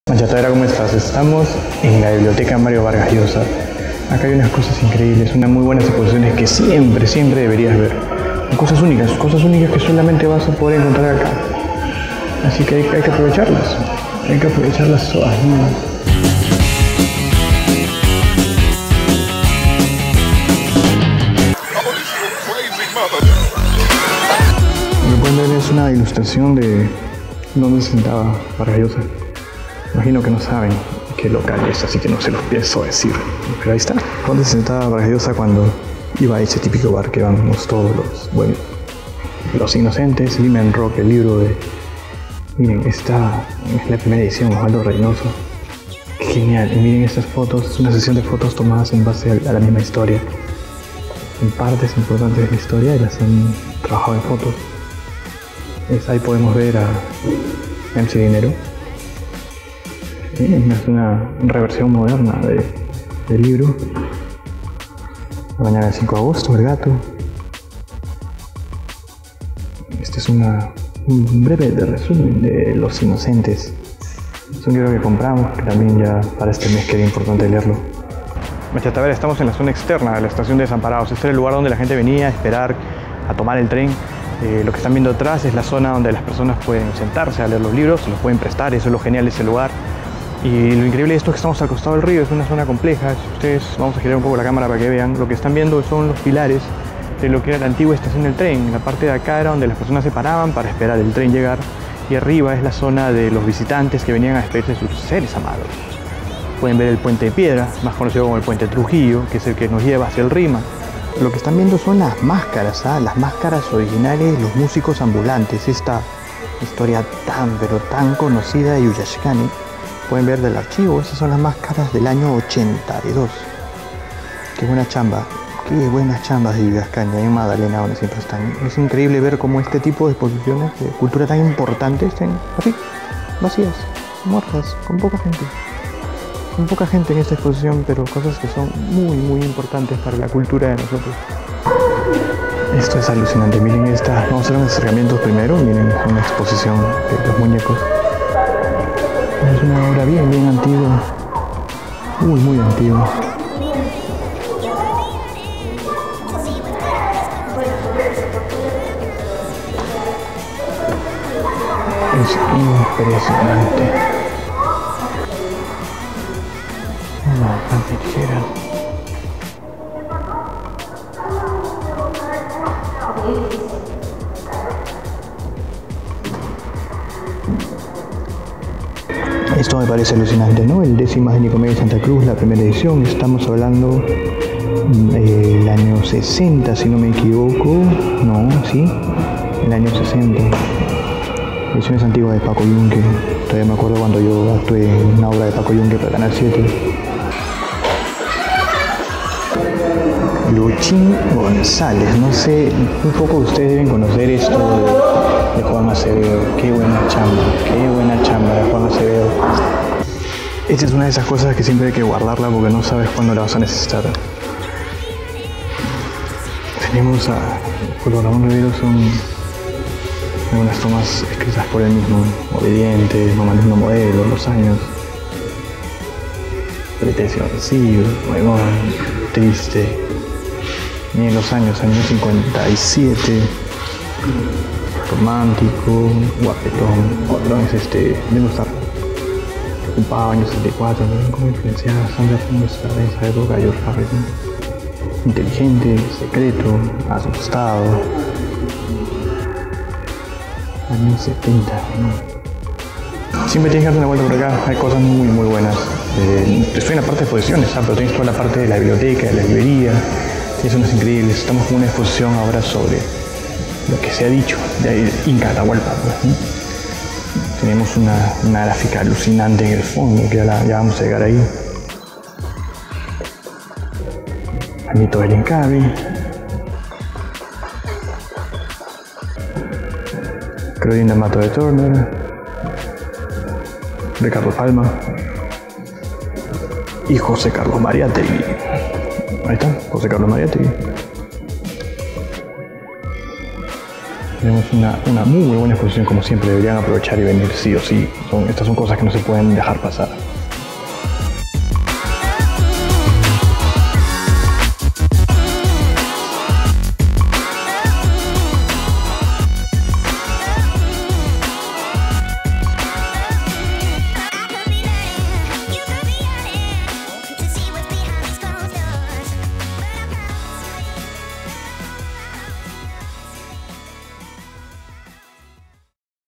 Casa de la Literatura Peruana - Me Haces la Taba
Visitamos la casa de la literatura peruana para conocer la nueva exposición sobre los pilares del arte limeño. Marionetas, grandes ecitores, libros, comics y como siempre harta ManchaTabera.